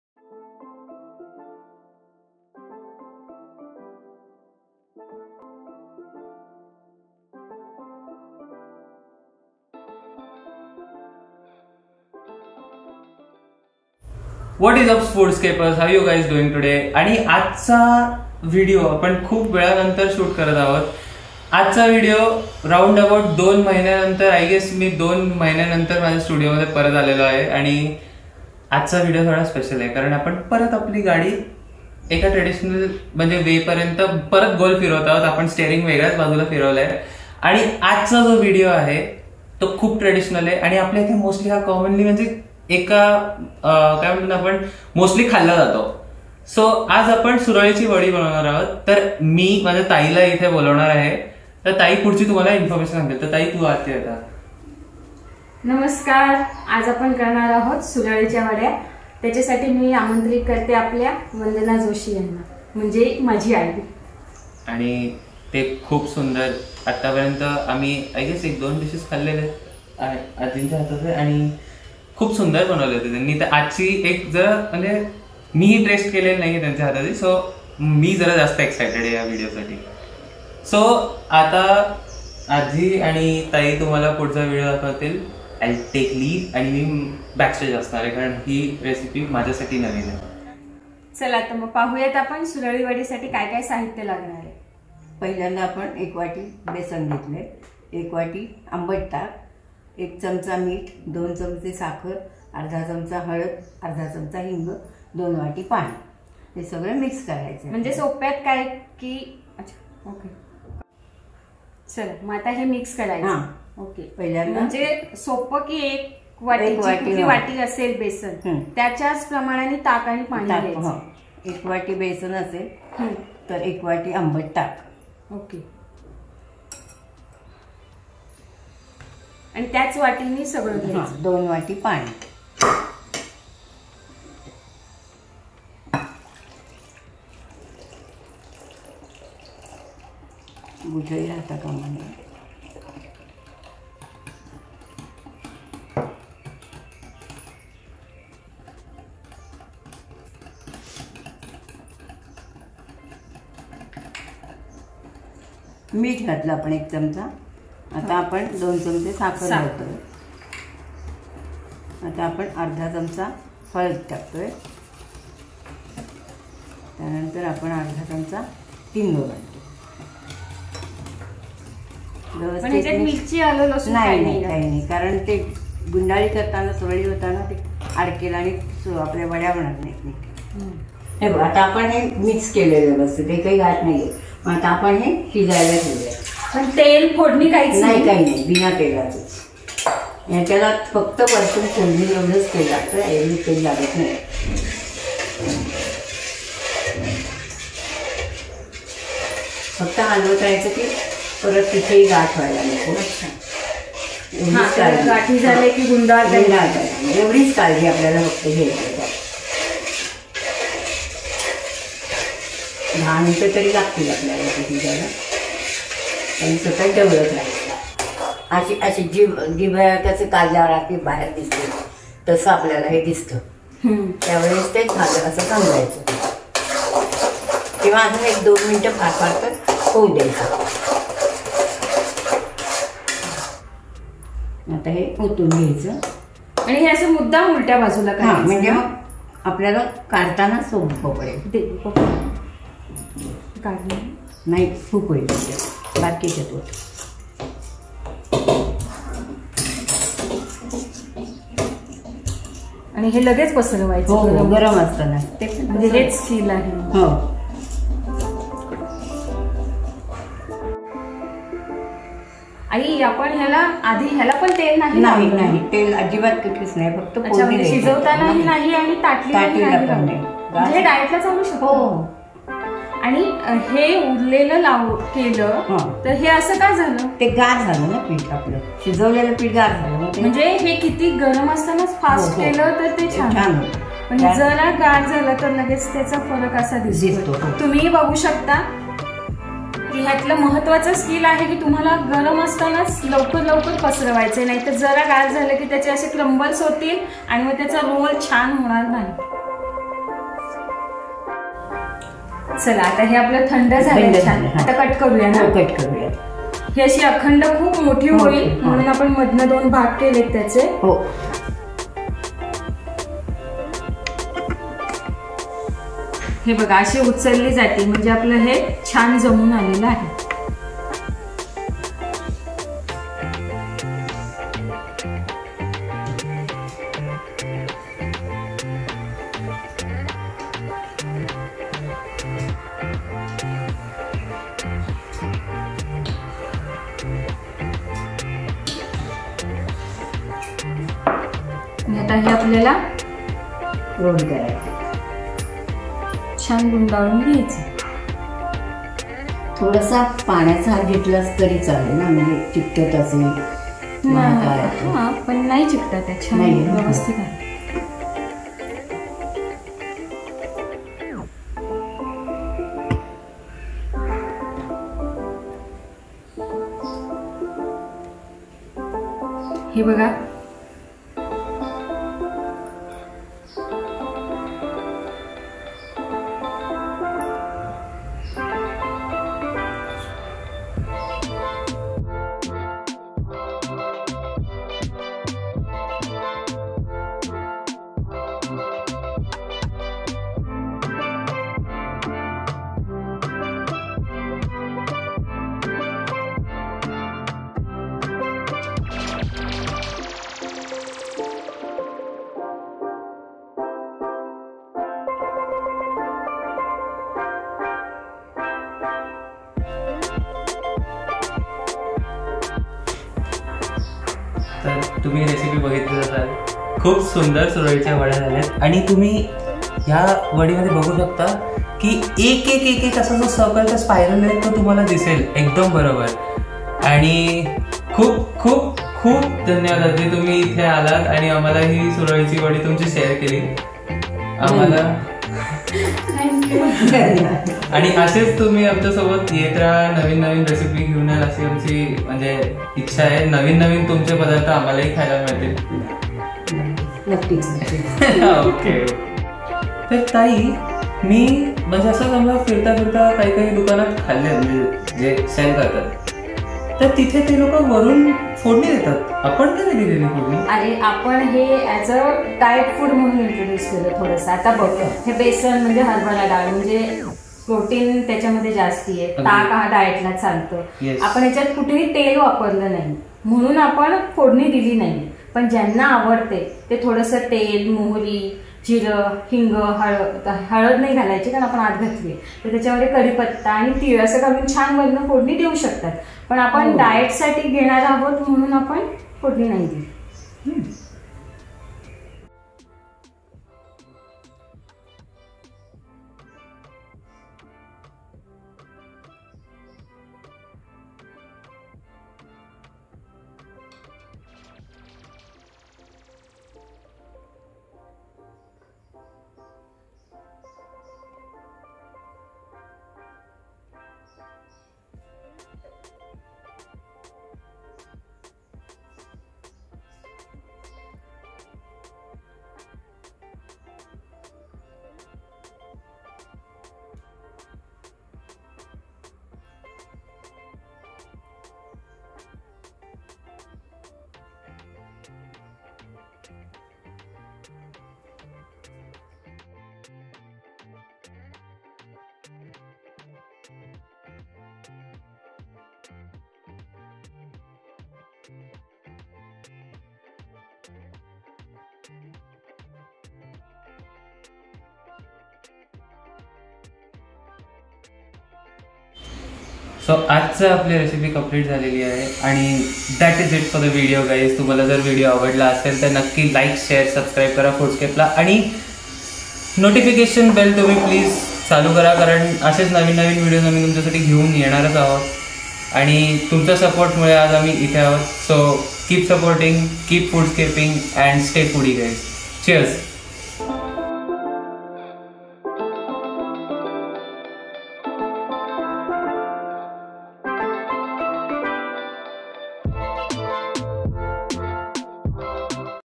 SURALICHI WADI | AAJI CHI SPECIAL
WHATSUP GUYS WELCOME BACK TO FOODSCAPE

TODAYS RECIPEE IS VERY KNOWN TO ALL OF US ANDHAS SPECIAL PLACE IN EVERY MAHARASHTRIANS HEART

SURAALICHI WADI IS A DISH MADE FROM DAAL AAND THUS CONTAIN HIGH AMOUNT OF FIBER AND PROTEIN
WHEREAS USING A SOUR BUTTERMILK ADDS INN THE TANGY HINT AND MAKES THE WADI MORE LIGHT AND EASY TO DIGEST